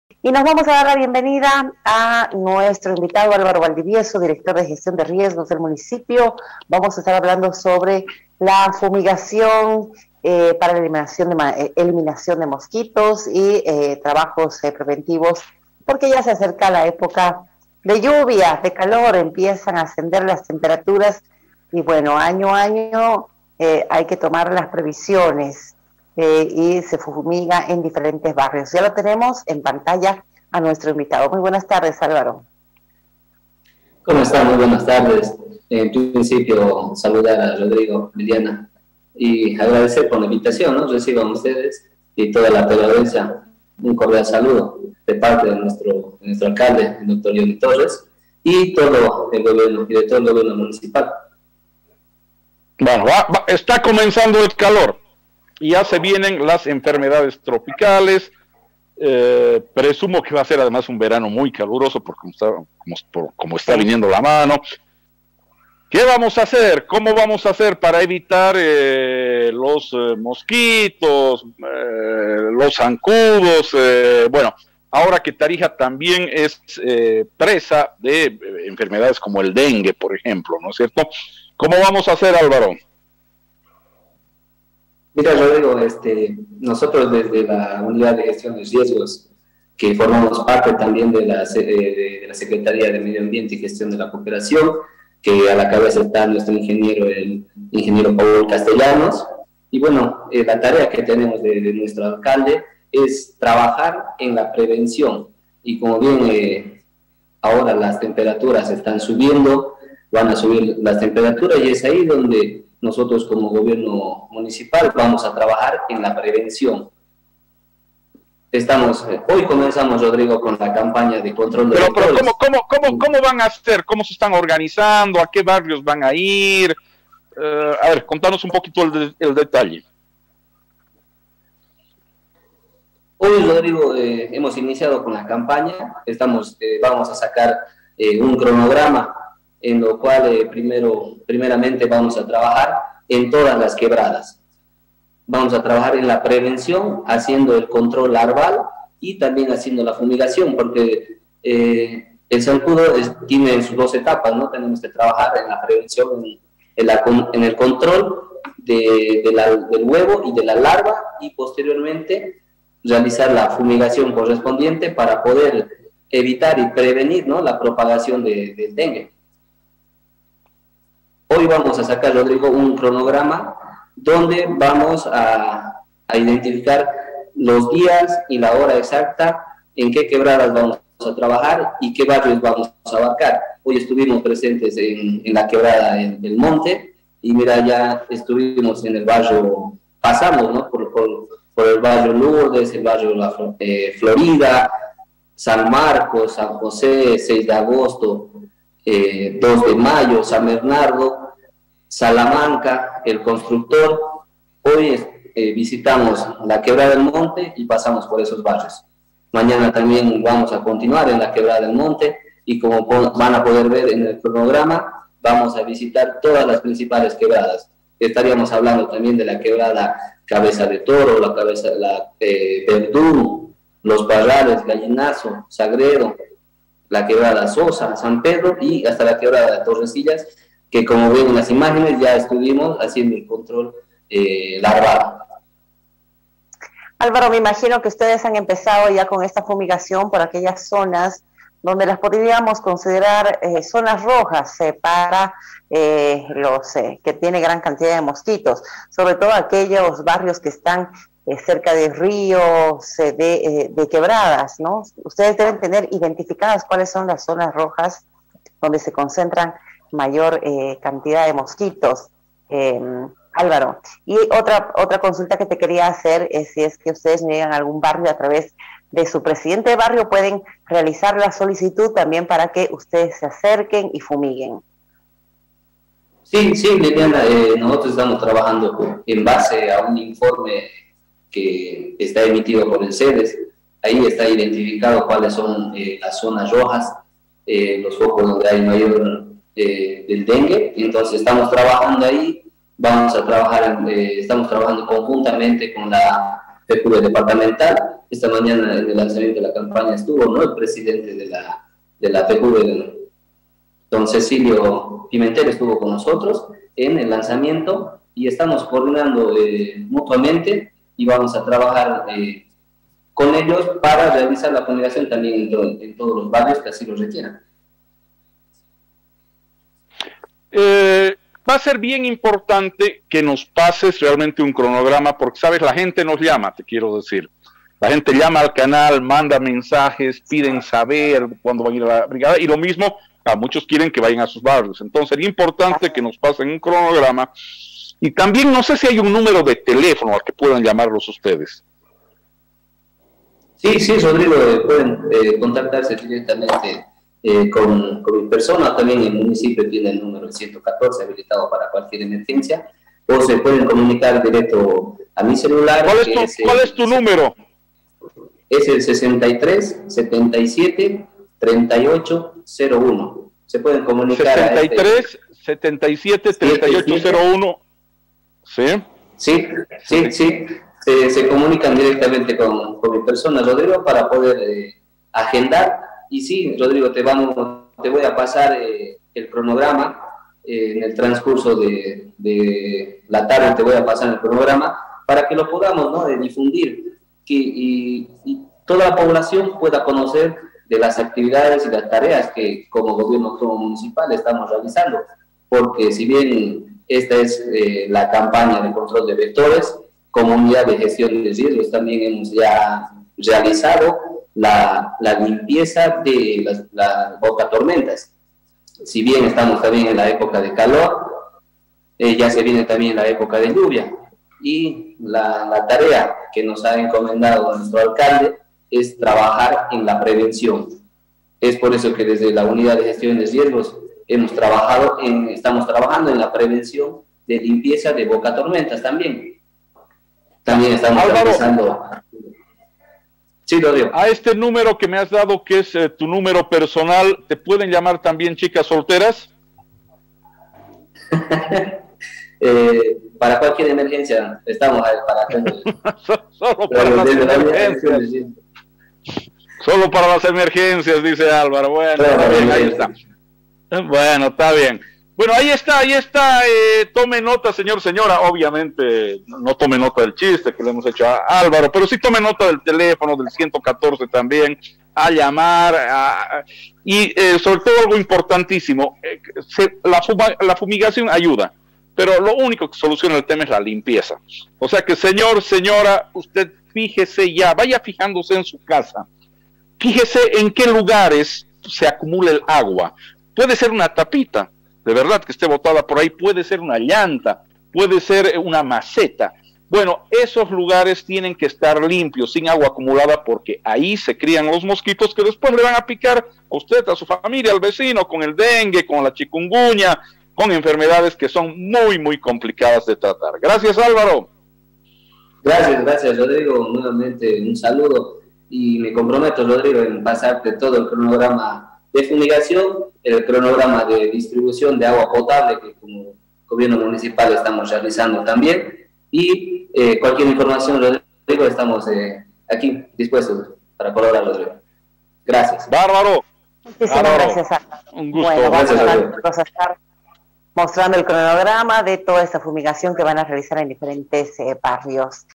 ÁLVARO BALDIVIEZO – DIR. MUNICIPAL DE GESTIÓN DE RIESGOS
BALDIVIEZO: “ESTAMOS IDENTIFICANDO LAS ZONAS ROJAS PARA EL CONTROL LARVARIO DE VECTORES Y REALIZAR LAS FUMIGACIONES”